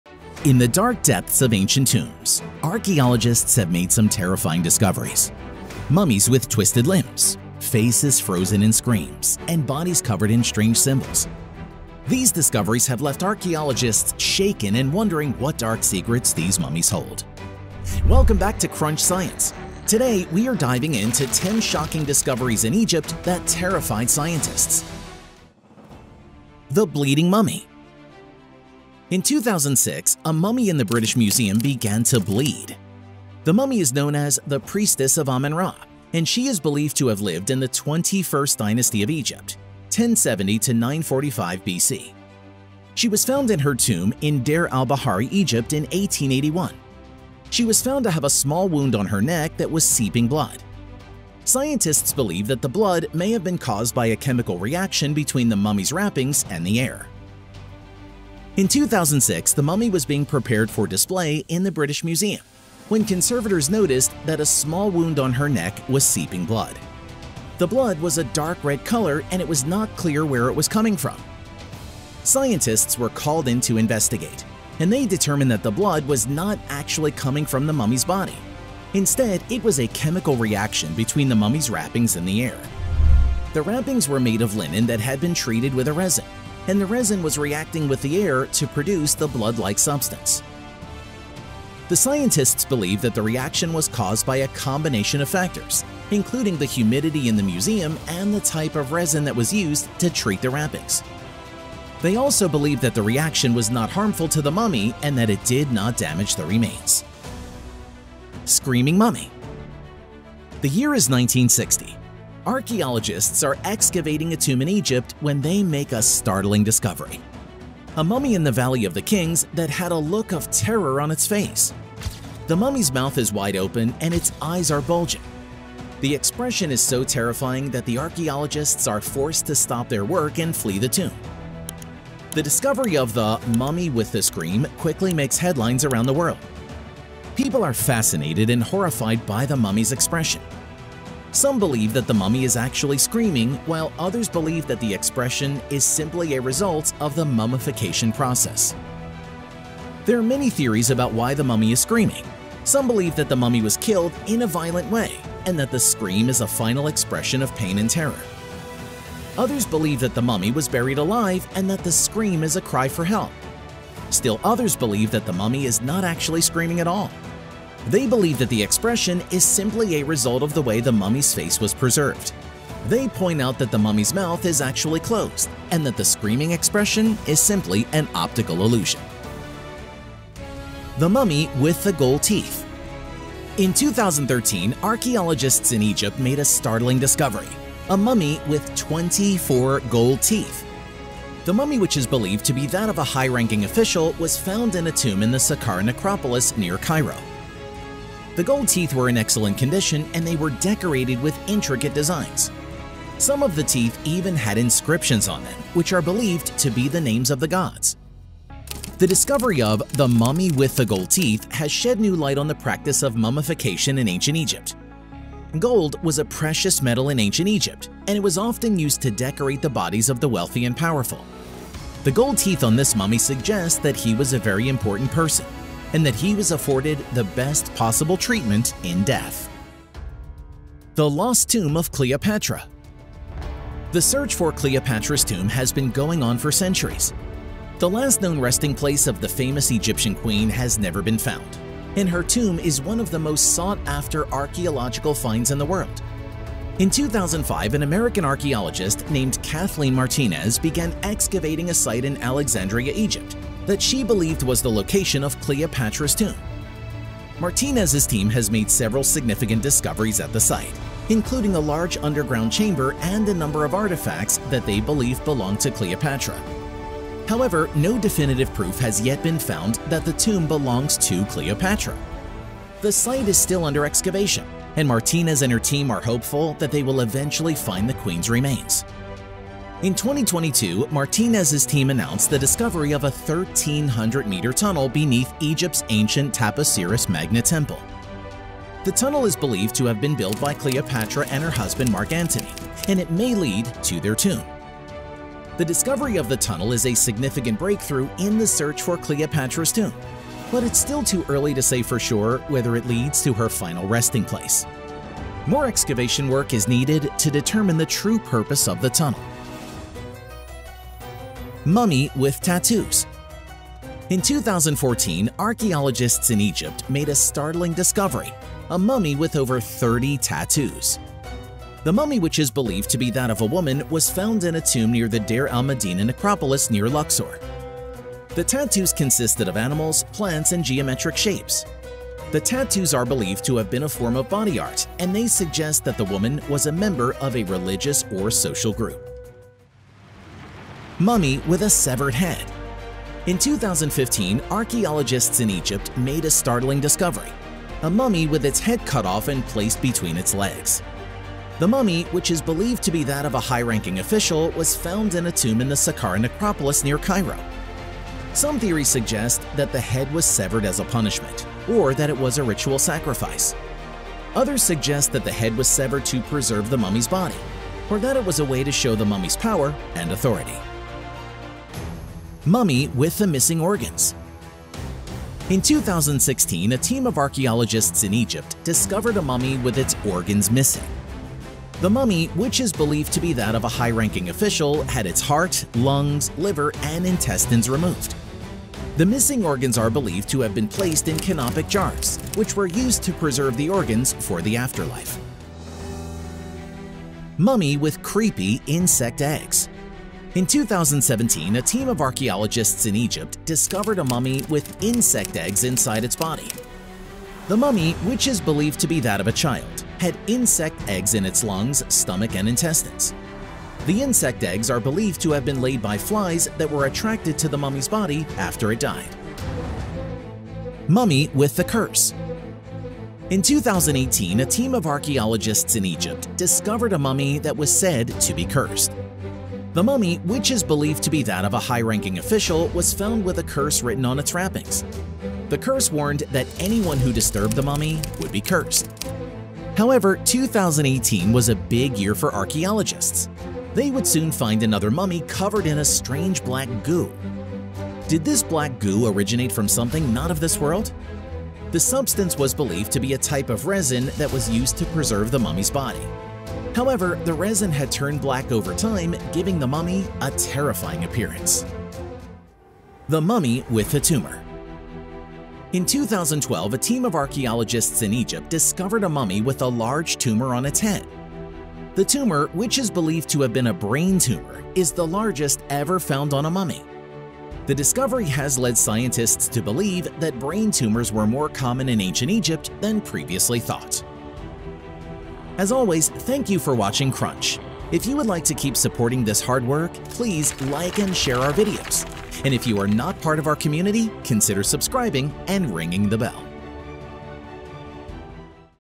10 SHOCKING Discoveries in Egypt That TERRIFIED Scientists
Brace yourself as we unveil 10 shocking discoveries that have left scientists terrified and perplexed. Witness the haunting secrets of the Bleeding Mummy, the mysterious Screaming Mummy, the eerie Mummy with Gold Teeth, the elusive Lost Tomb of Cleopatra, and the bone-chilling Mummy with Creepy Insect Eggs, among other astonishing finds. Get ready to delve into the depths of Egyptian history and uncover the dark and unsettling mysteries that continue to baffle the scientific community."